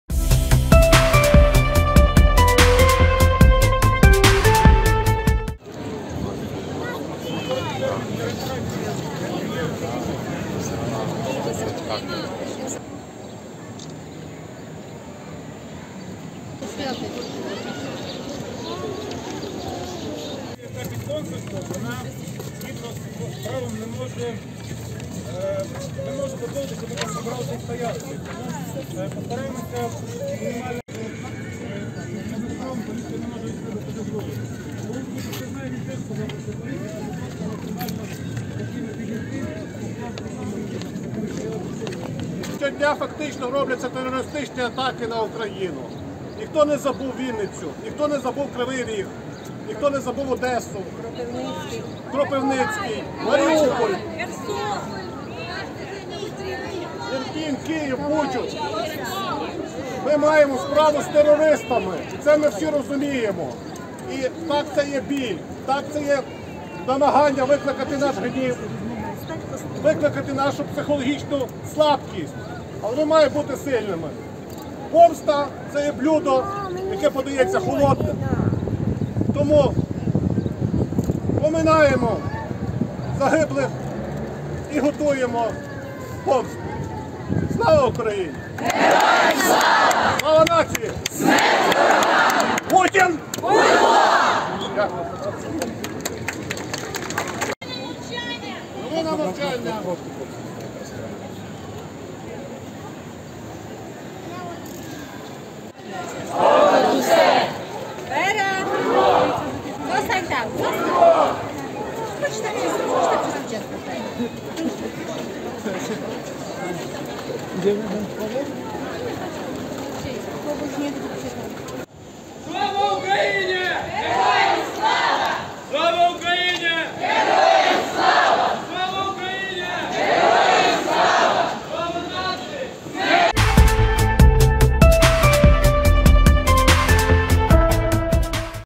Мітинг українців у Вроцлаві, через вбивство у Оленівці
Понад півтисячі українців мітингували у Вроцлаві через убивство українських військовополонених в Оленівці